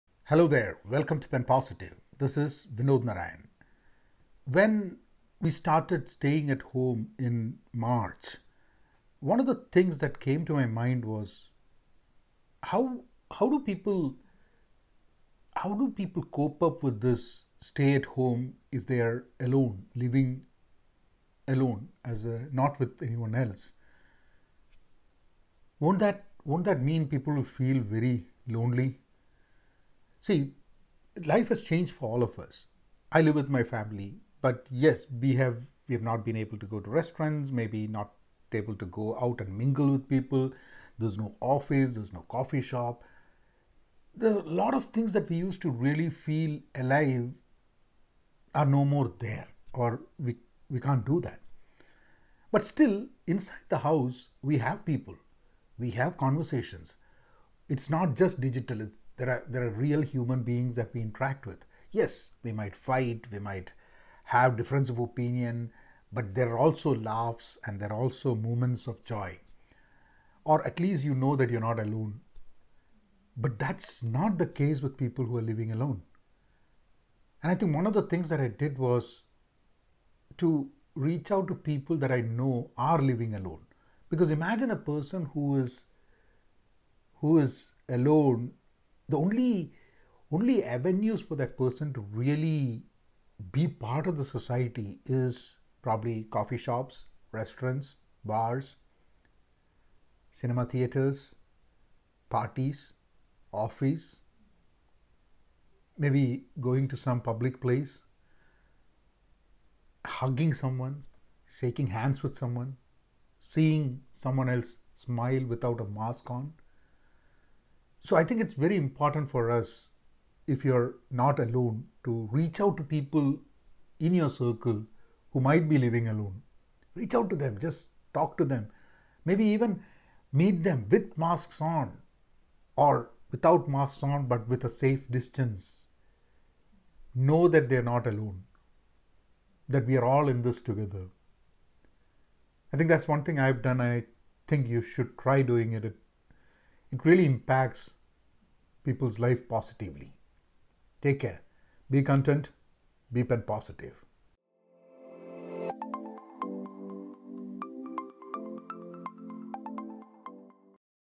Being Alone During Covid | Be Content; Be Penpositive
Life has changed for all of us but as we were few days into the ‘Stay at Home’ life the thought that really struck me was of people who might be living alone. And I did start connecting with a few.

If I have missed anyone please ping me and if you are in town and alone let us catch up...

The fact is that we are all in this together...
Be Content; Be Penpositive !!
Vinod!